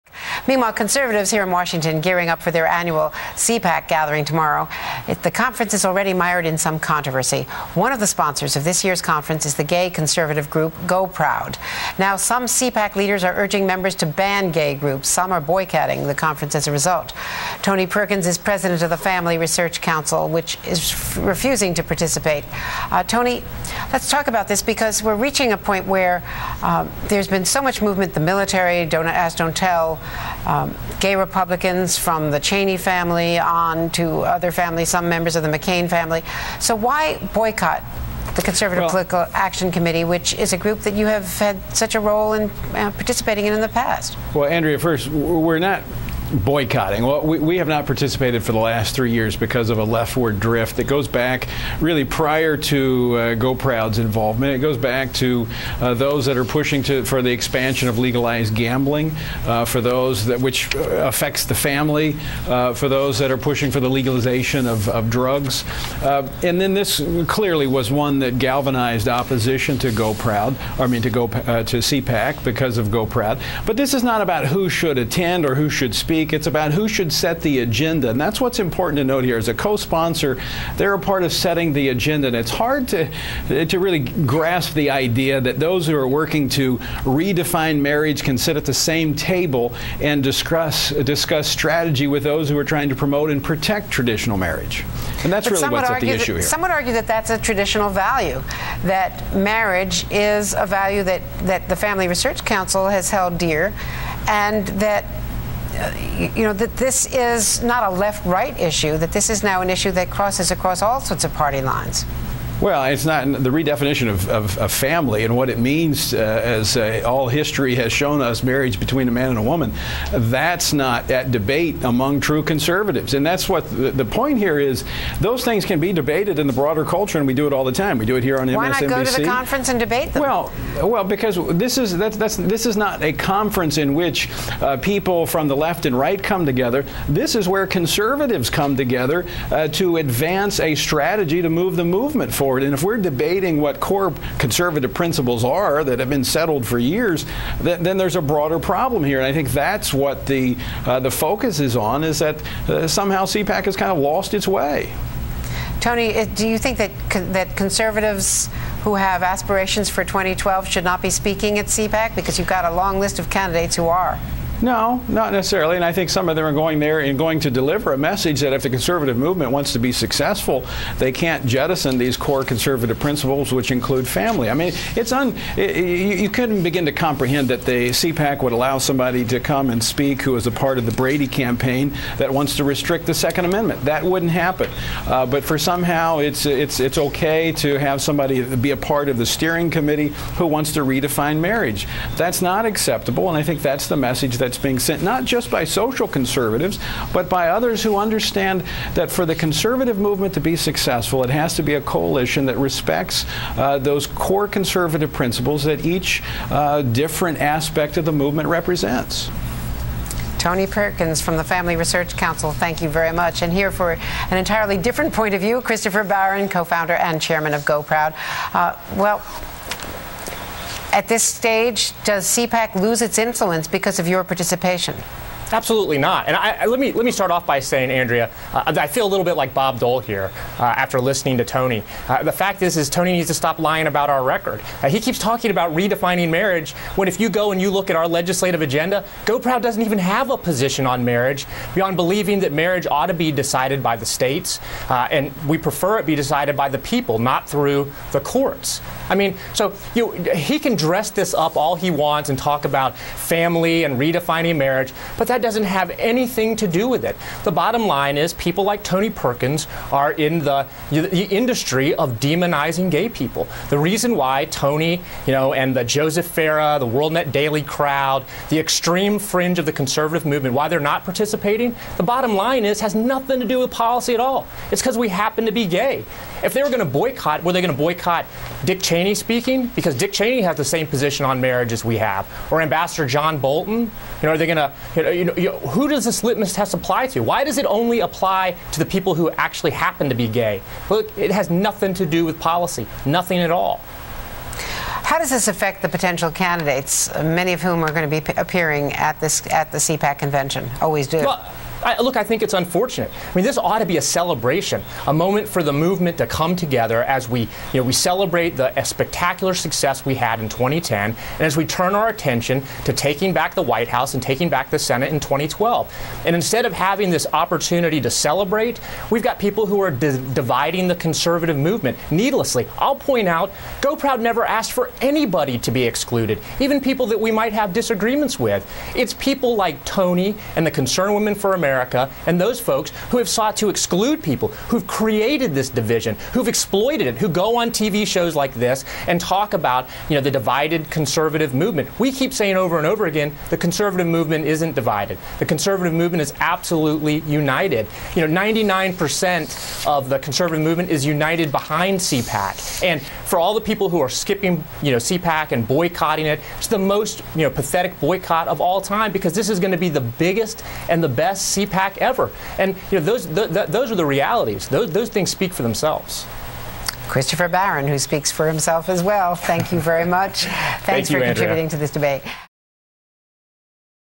FRC's Tony Perkins Vs. GOProud's Christopher Barron On CPAC Boycott
Family Research Council among those boycotting CPAC over GOProud's attendance.